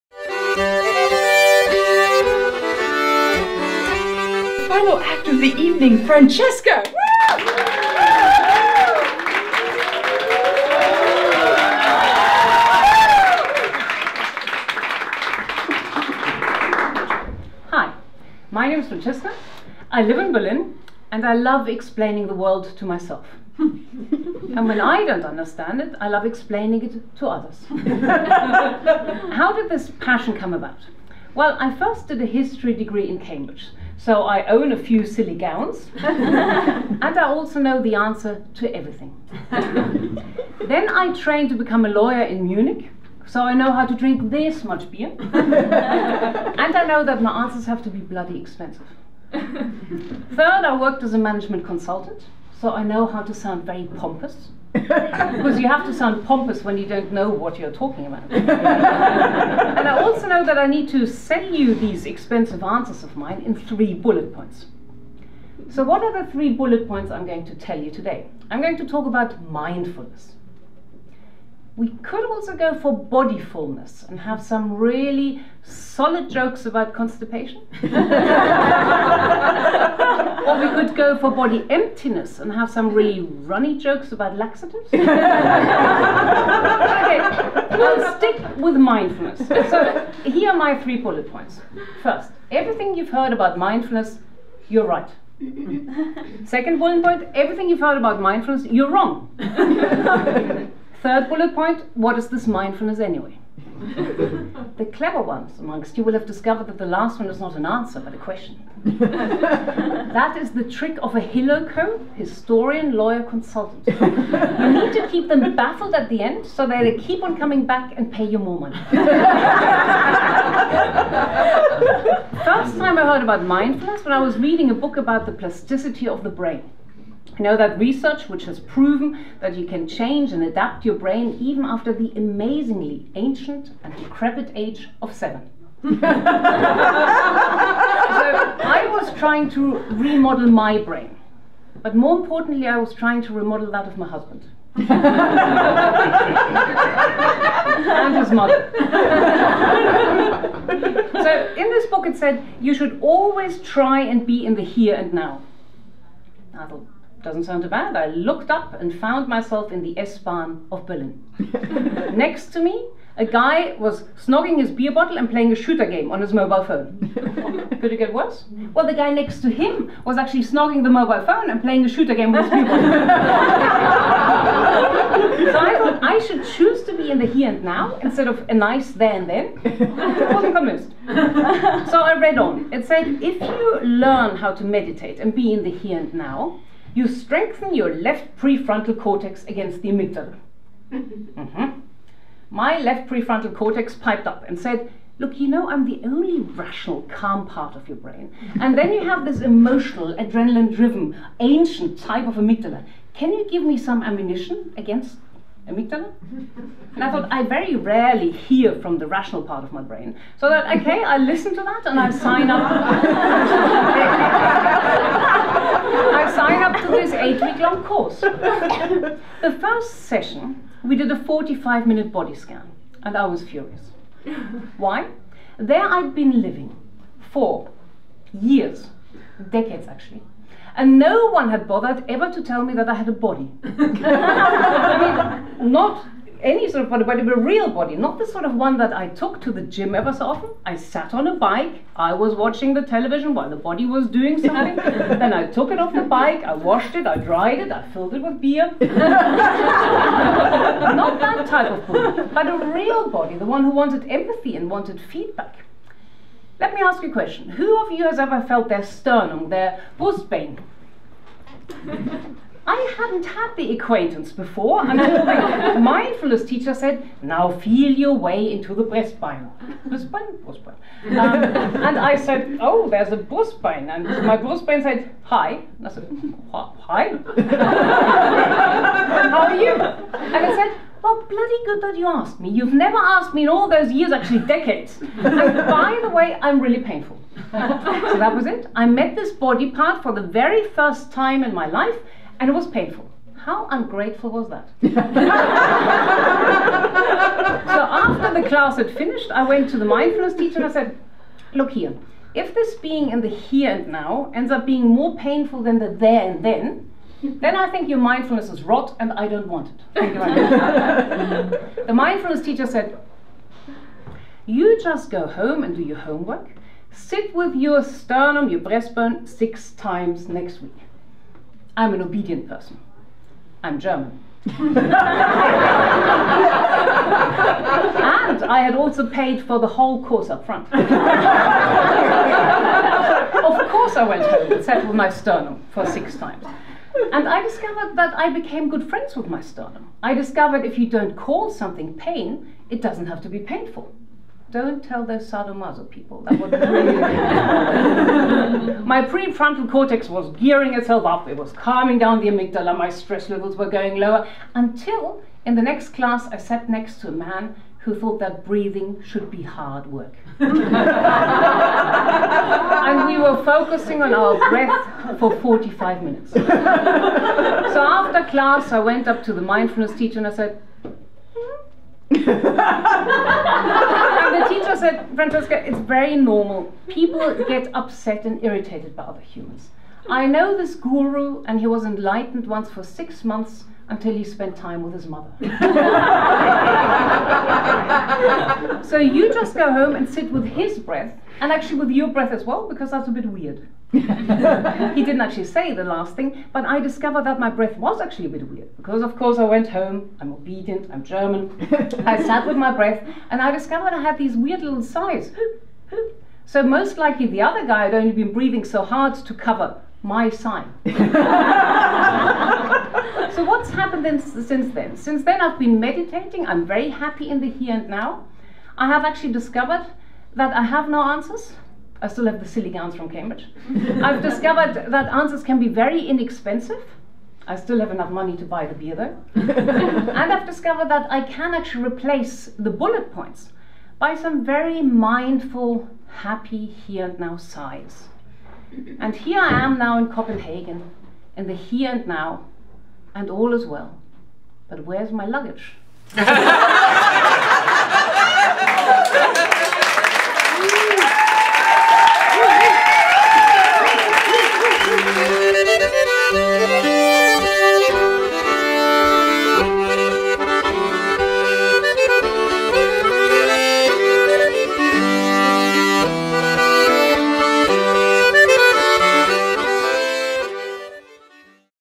Sustainable Stand Up Copenhagen - Franziska Frank
This is the set from the lovely Franziska Frank, mind opener for managers, educational entrepreneur and DIY brain plasticist.  Also a participant on the Sustainable Stand Up Copenhagen course.  This show was on March 18, 2018.  If you're interested in our global project and courses, please go